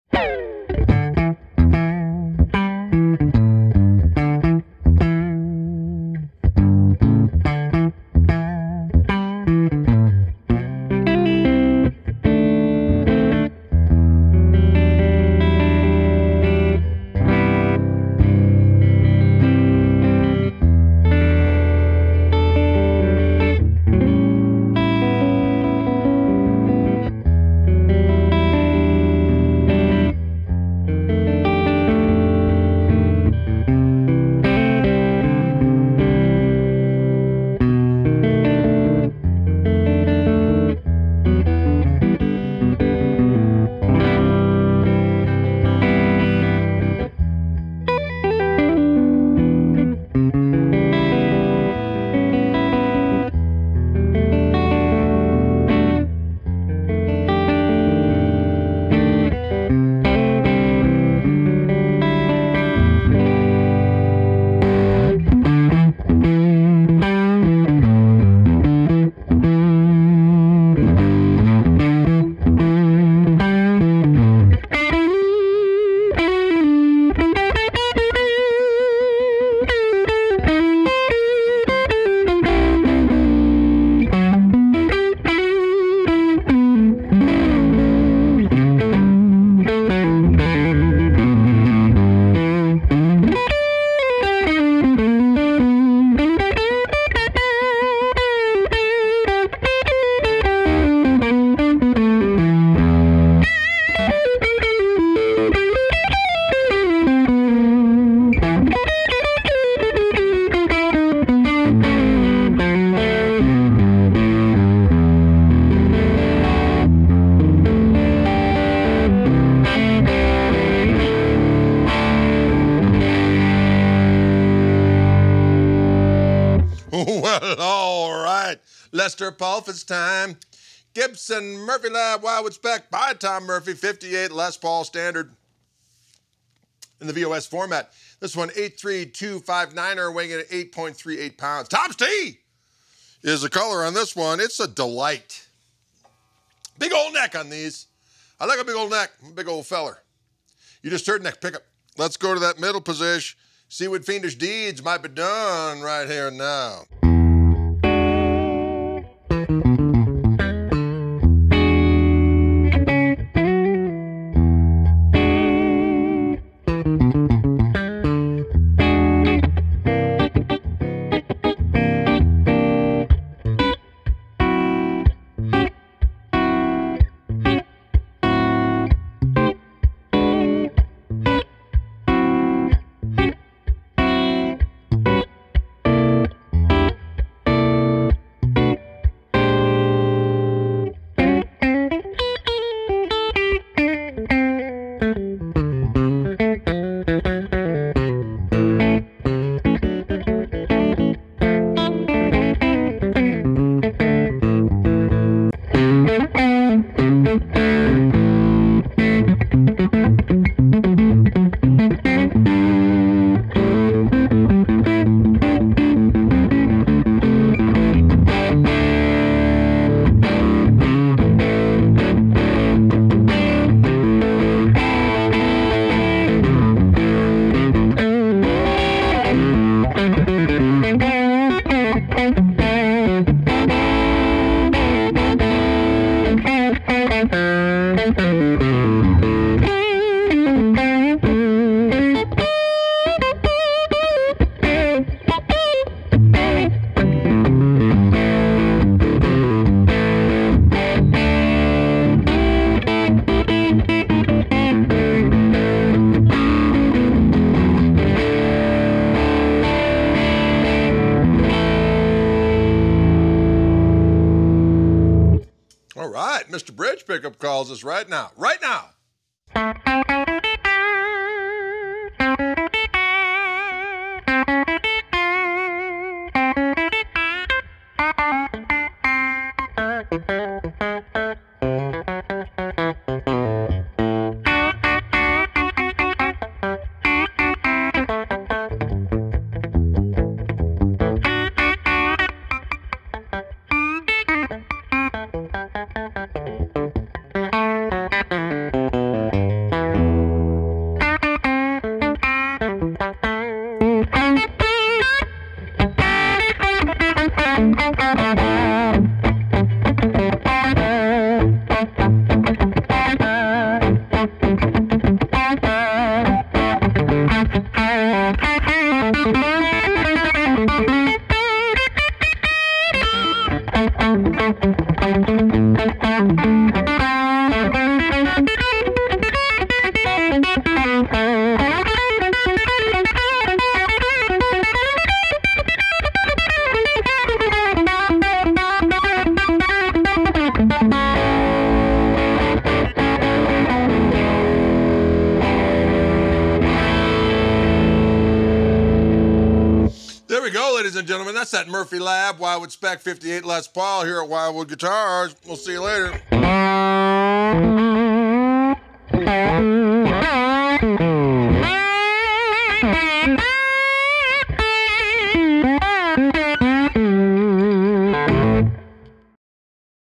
Gibson Murphy Lab Wildwood Spec by Tom Murphy 1958 Les Paul Standard - VOS • SN: 83259
There are 7.764 billion people on this beautiful ball of water and dirt that we call home, and hardly any of them know Gibson guitars better than Tom Murphy. Tom is a legend in the world of guitars, and instruments bearing his name have acquired a deservedly sterling reputation due to their outstanding tone, jaw-dropping looks, and stunning vintage accuracy. But, he wasn't always a rockstar of lutherie. In fact, he worked as a professional touring musician for many years, and during that time he became intimately acquainted with vintage guitars: how they sound, how they feel, and how they look. After deciding to stop touring, Tom got a job as a finish specialist at Gibson and eventually became a project manager in charge of the first reissues of the 1959 Les Paul standard.

For a while, Tom also gained valuable experience working as a luthier to the stars, and his client list included luminaries like Jimmy Page, Dickey Betts, Joe Perry, Billy Gibbons, Eric Clapton, Peter Frampton, and Gary Rossington.

Now, things have come full circle, and our friends at Gibson have handed Tom the keys to a brand-new institution of sonic savagery within the Gibson Custom Shop: the Murphy Lab. Tom and his team of skilled craftspeople have one mission, and one mission only: to create the most ridiculously awesome recreations of vintage Gibson guitars that the world has ever seen.